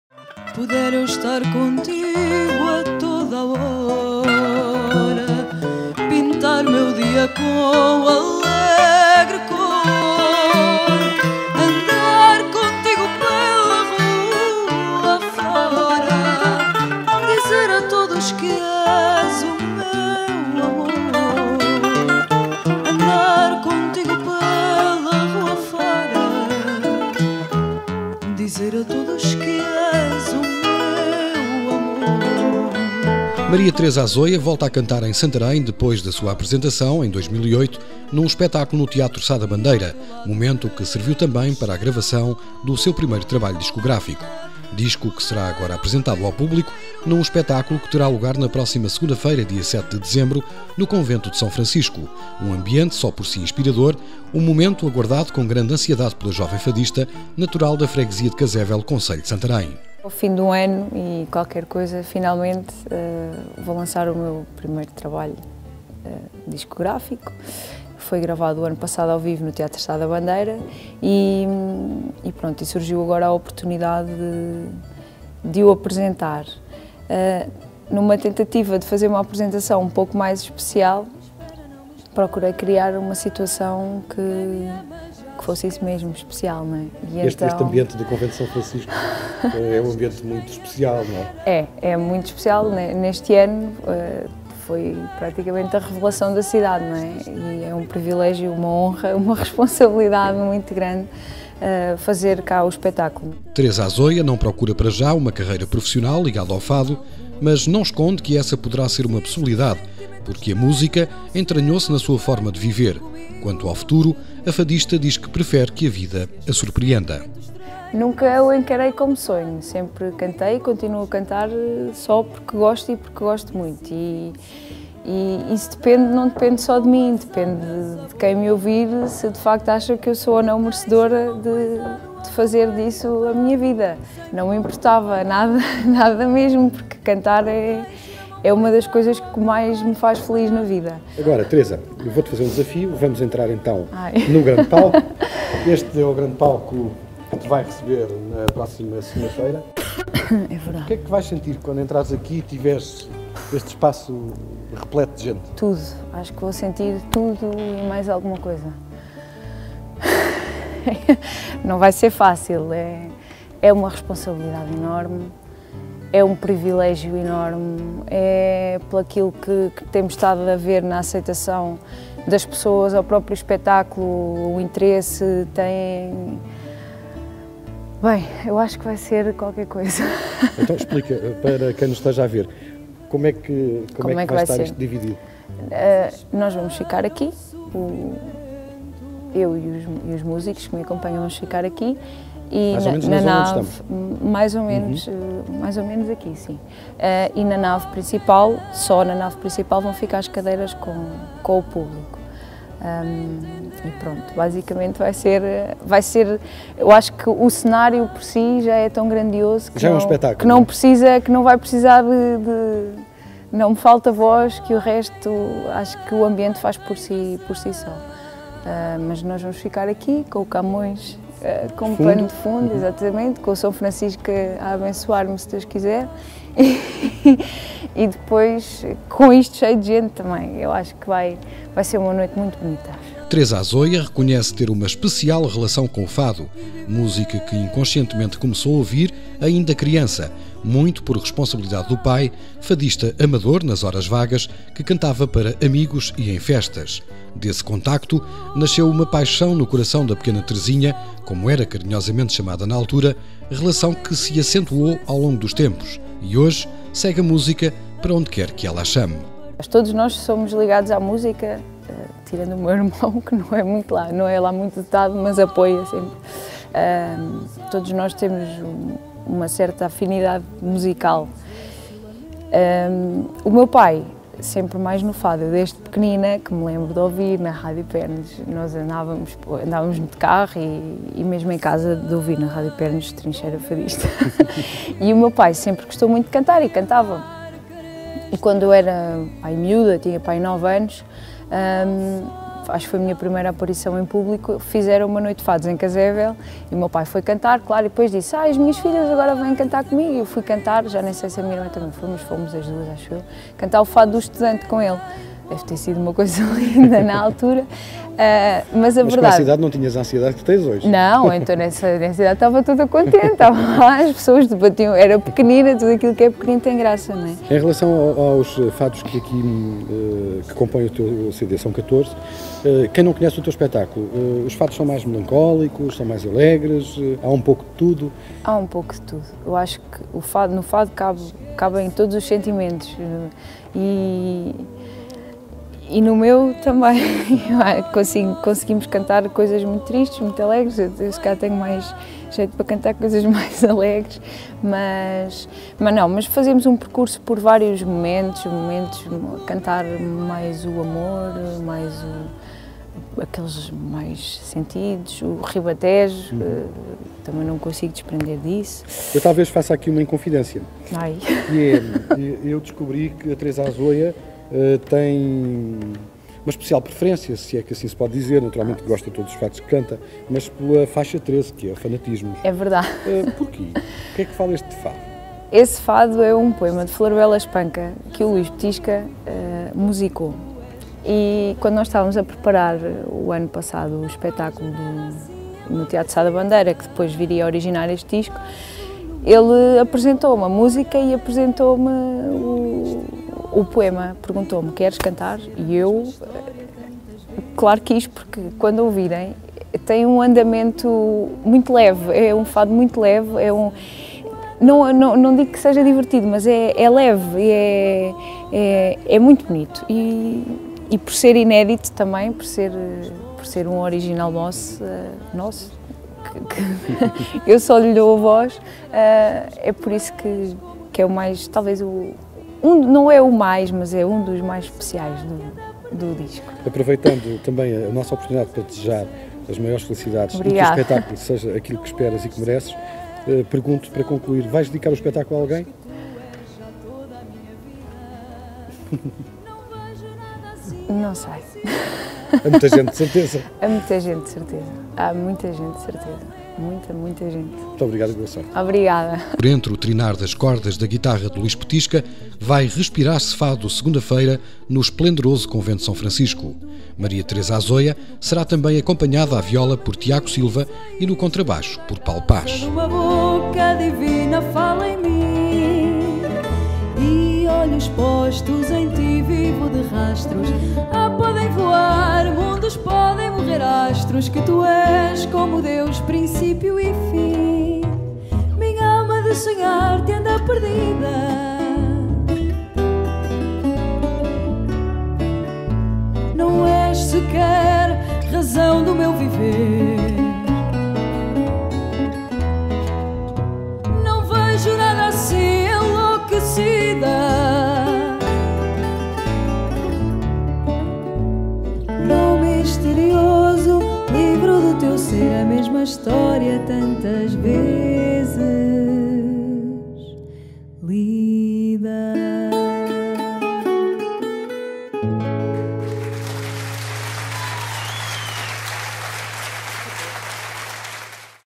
Maria Teresa Azoia canta em Santarém
Fadista actua no dia 7 de Dezembro no Convento de S.Francisco